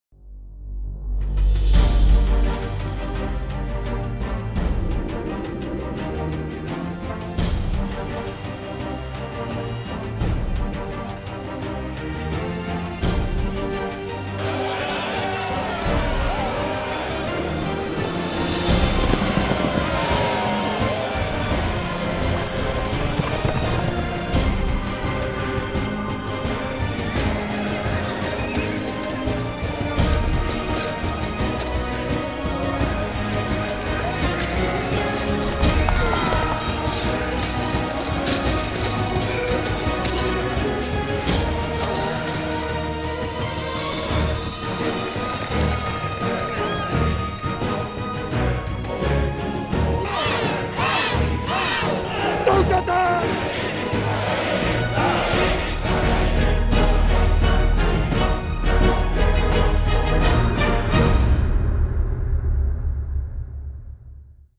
Great Roman Games, Nimes, France : 23 and 24 april 2011
The biggest historical reenactment in the south of France : live the roman games in the antique arena of Nimes.